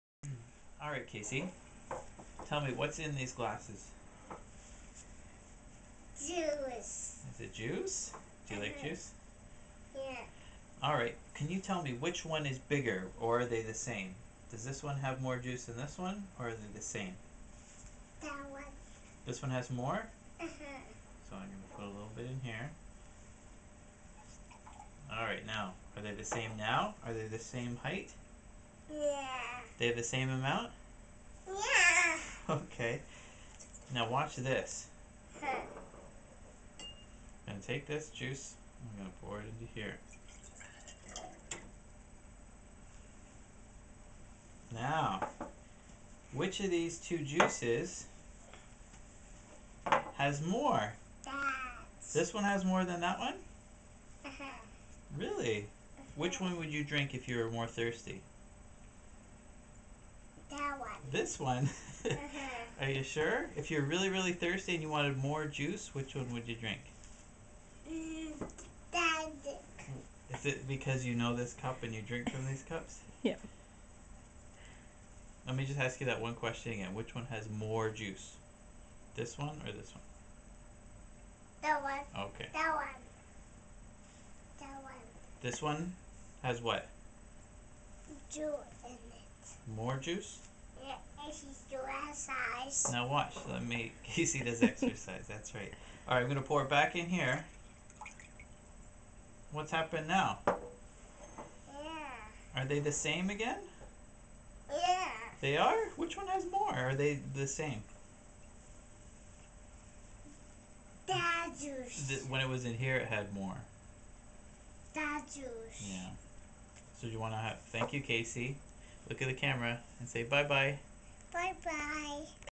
Liquid Conservation Task
Casey, 2 years and 2 months old, attempts the classic Piaget conservation task. The difficulty she had with the question "which would you drink if you were more thirsty" comes from the fact that she drinks from the smaller wider glass frequently, whereas she has never used or seen the tall glass.